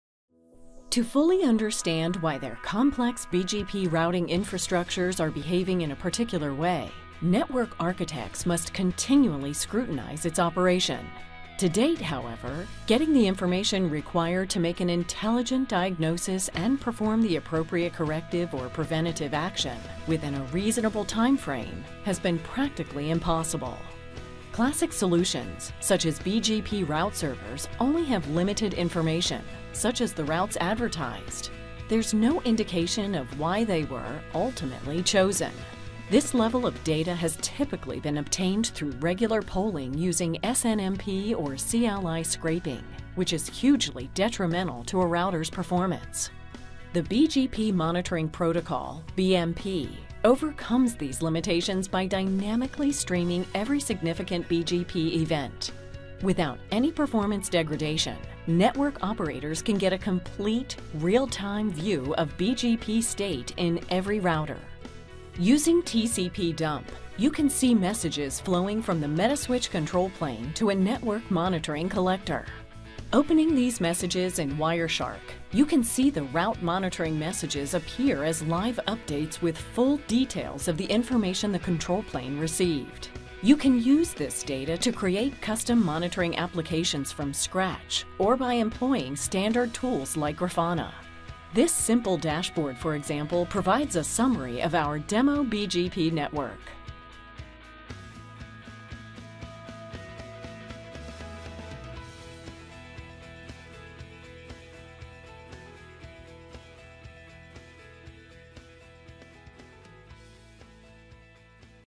Implementing the BGP Monitoring Protocol (BMP) in your NOS to track BGP network state
In this short video, we take a look at the Metaswitch implementation of BMP -- available as part of the Metaswitch Network Operating System (NOS) Toolkit -- and how it can be used in a network to provide a real-time graphical view of the operation of a BGP network infrastructure.